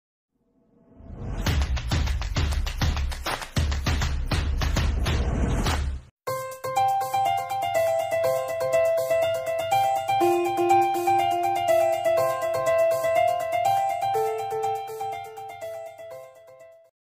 Android AirDrop First Look & User Guide.
Android Air Drop is launched recently and its pretty fast though.